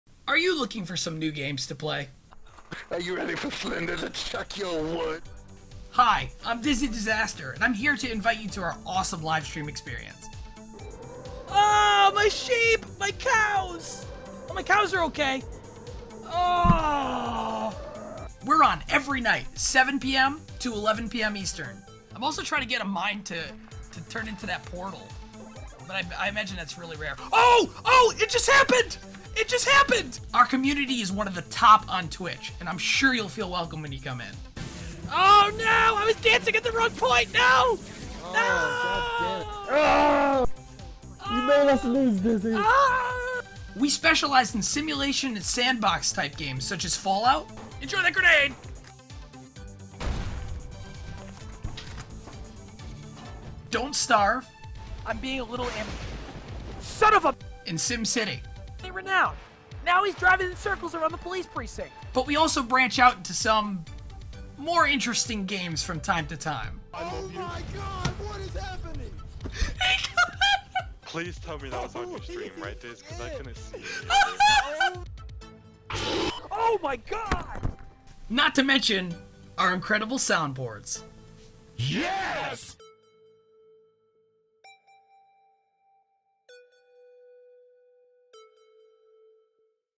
Dizzy Dizaster on Twitch - 90 Second Promo (2014)
This is a 90 Second Dizzy Dizaster Promo we did for Twitch Weekly. I decided to make it the channel trailer for October!

DizzyDizaster is an active streamer on TwitchTV that loves audience interaction! and toss us a follow!